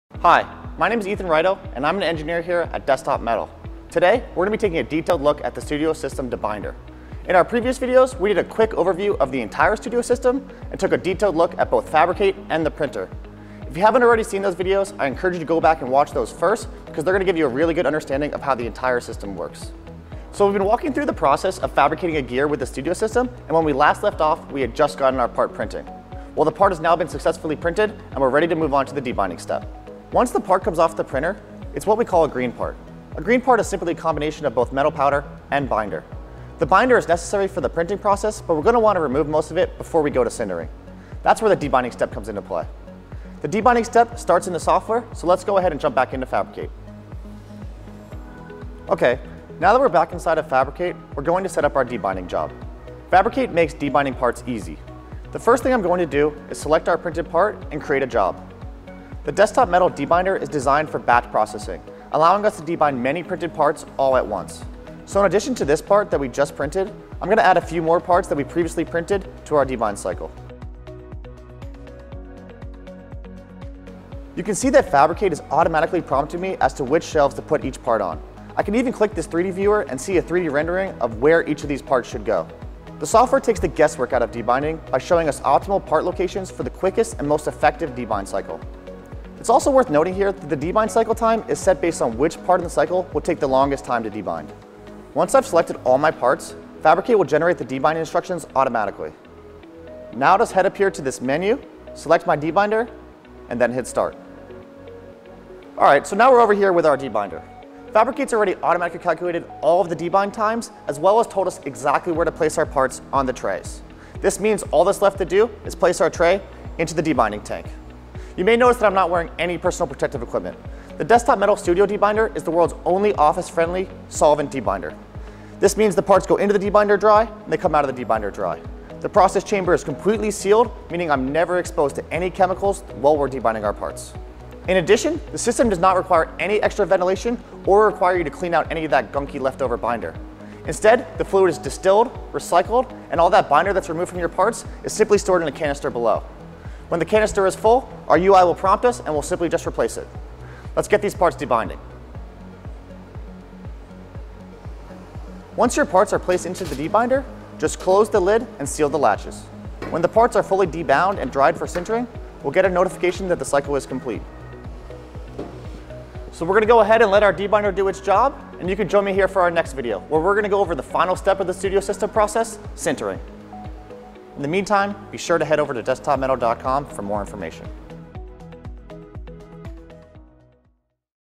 Introduction to the Studio System Debinder
Desktop Metal's Studio System uses a 3 step process that includes a step for debinding. In this video, we walk through how the debinder works and some key features it has.